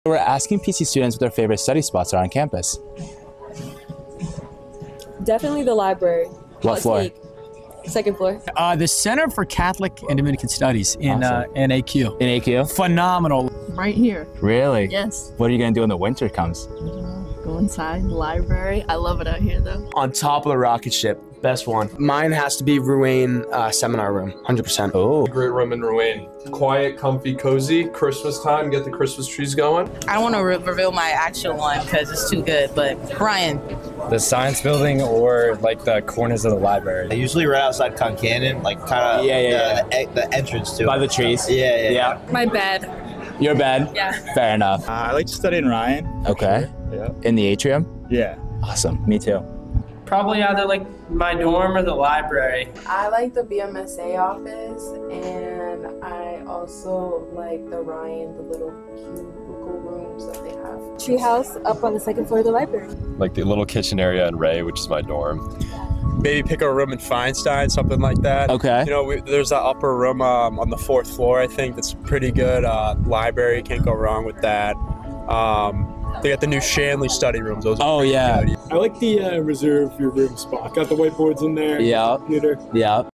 Friar On The Street: Best Study Spots on Campus
Midterms are on the horizon, so we asked Friars to share their favorite study spots. Do you like to study somewhere that wasn't mentioned? Not sure where one of the featured spots is located? Drop a comment and let us know!